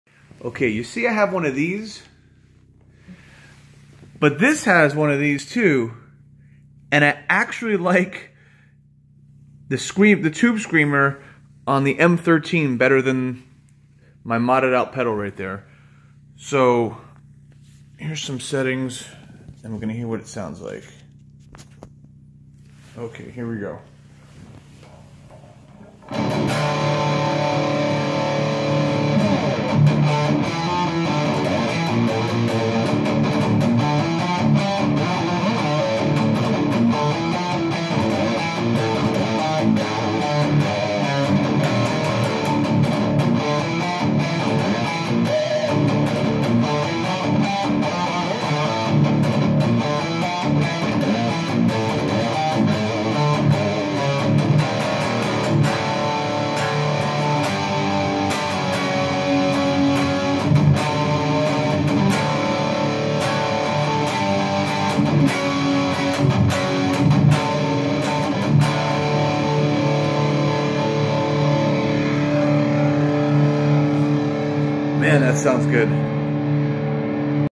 Line 6 M13 screamer Mesa Boogie Single Recto
Quick riff using the M13 screamer to boost a m/b single rec